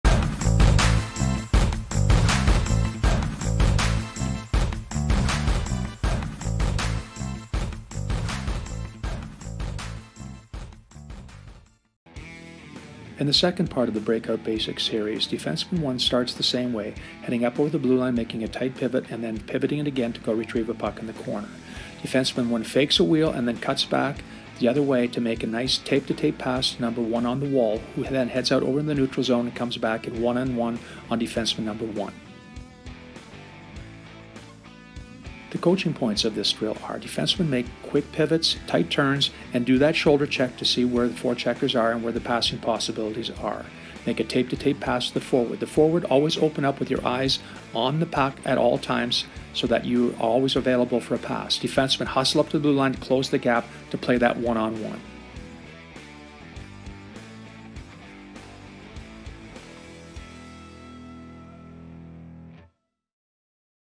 Break Out Basics 1vs1
In the second part of this teaching progression, we add a defender to create a 1vs1 after the initial break out pass.

Description:
D1 steps up over the blue line, pivots backwards, retrieves the puck, makes an evasive move behind the net and makes a break out pass to F1 on the wall. F1 skates out around the next players waiting in line and attacks back 1@1 against D1 who has gapped up to defend F1.

Key Points:
-Forwards time their pivot towards the wall and pivot to the wall once the D-man has committed to the cut back move.
-Forwards pivot towards the wall, forwards to backwards, always keeping their eyes on the puck.
-D-men fake wheeling the net before cutting back and making the quick, tape to tape outlet pass.
-After passing, D1 should move up quickly towards the blue line to set a tight gap on F1.